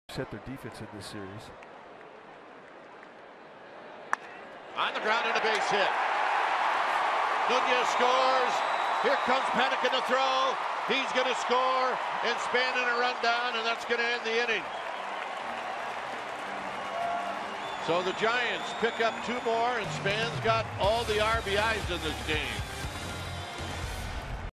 BAL@SF: Span drives in two more with second single
Denard Span plates Eduardo Nenez and Joe Panik with a single to right before being tagged out trying to advance to second